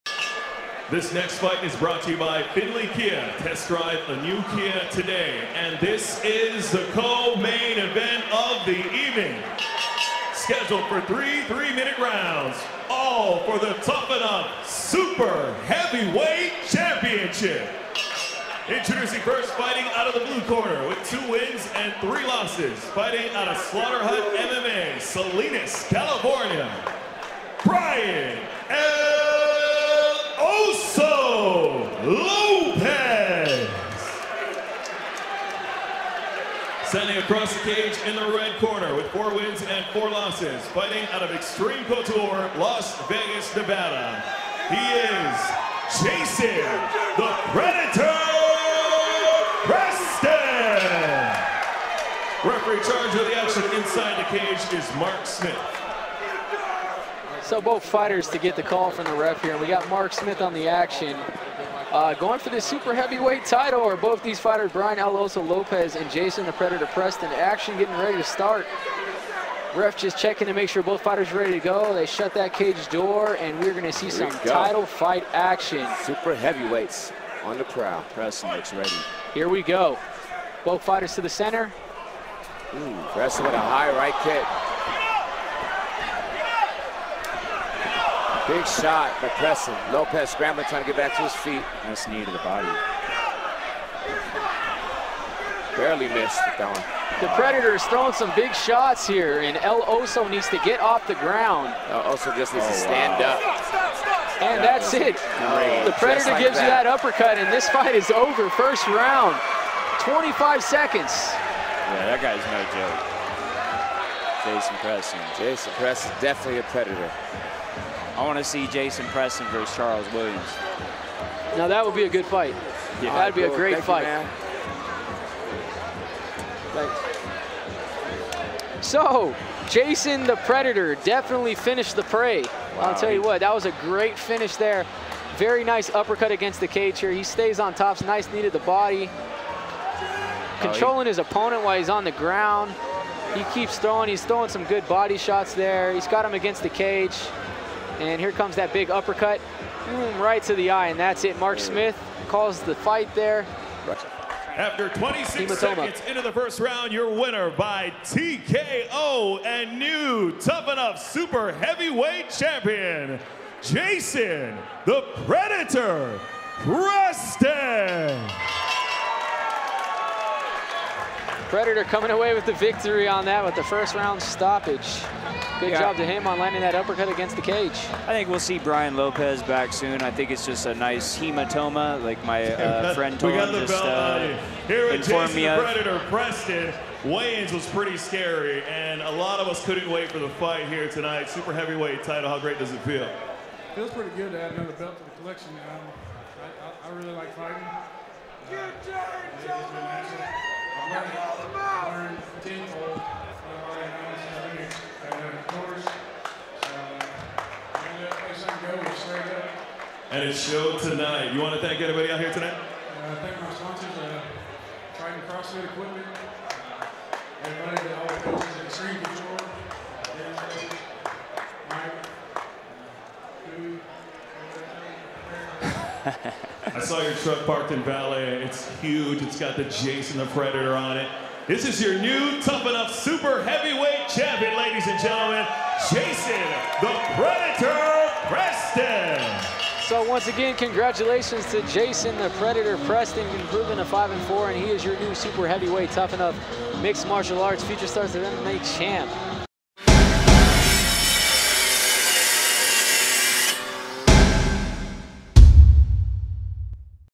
Brian Lopez vs Jason Preston
Tuff-N-Uff "Future Stars of MMA" at the Orleans Hotel and Casino in Las Vegas August 2, 2013.